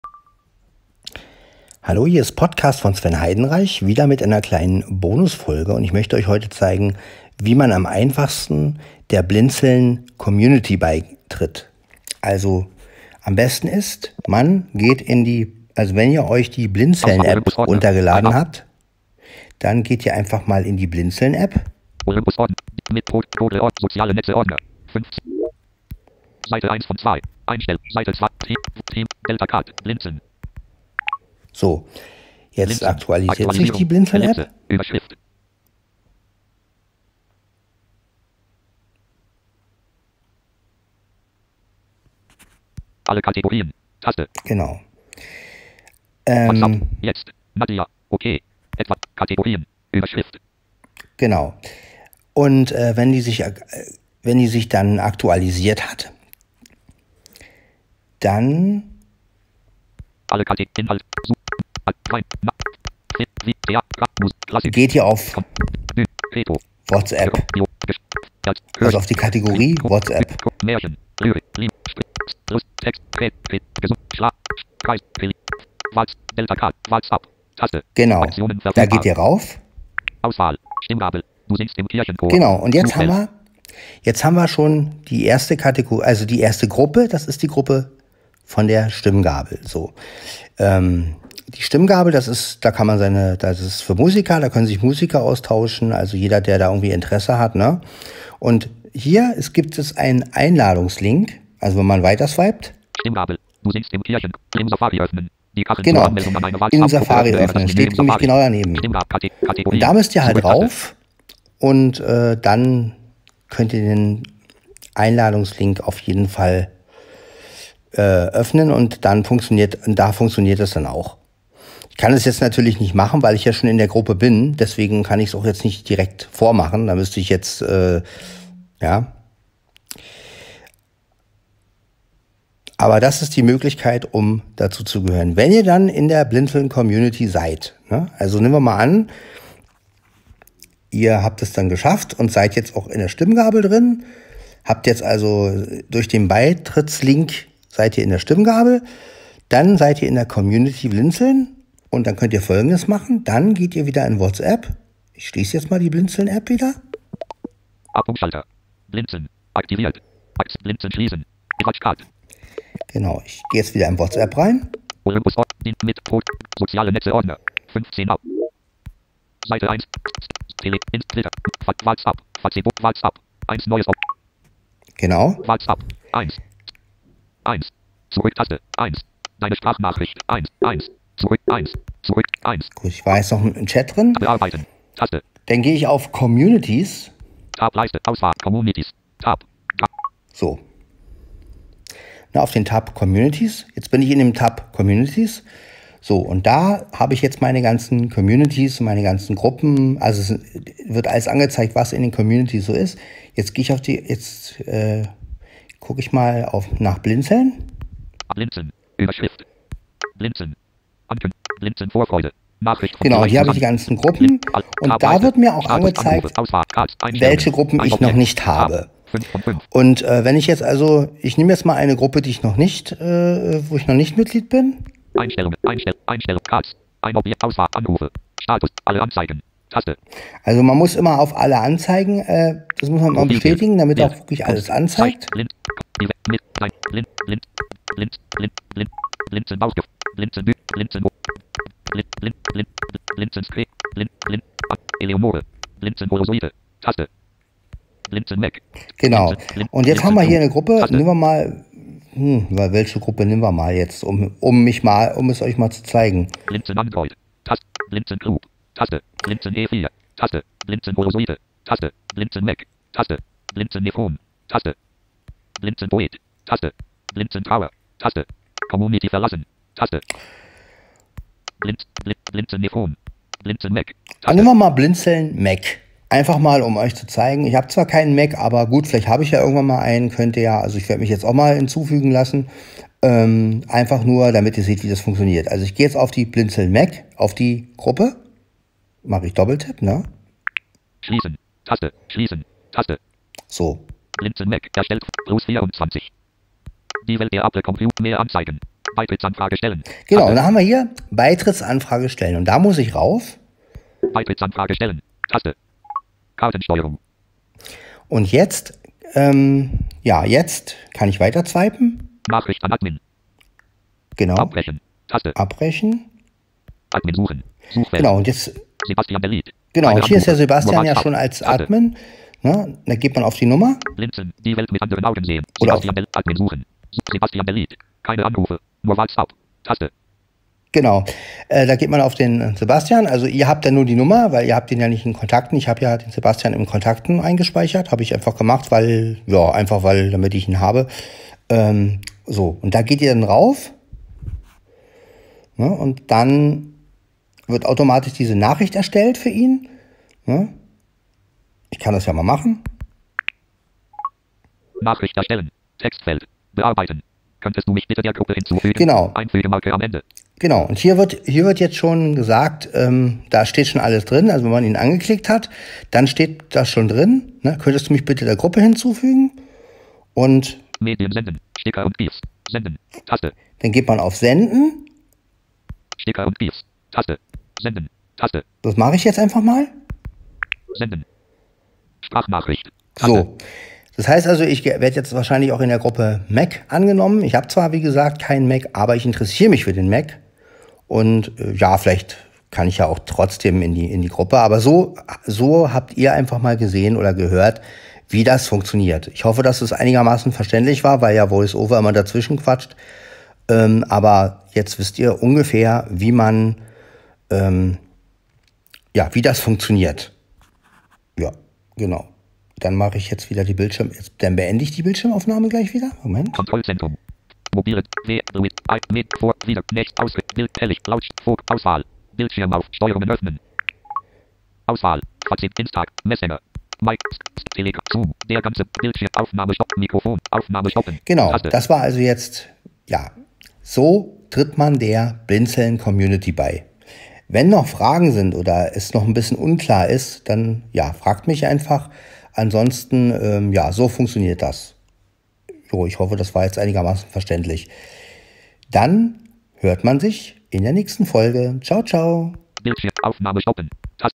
wie tritt man der Blindzeln community bei
in dieser Folge zeige ich wie man der blinzeln community Beitritt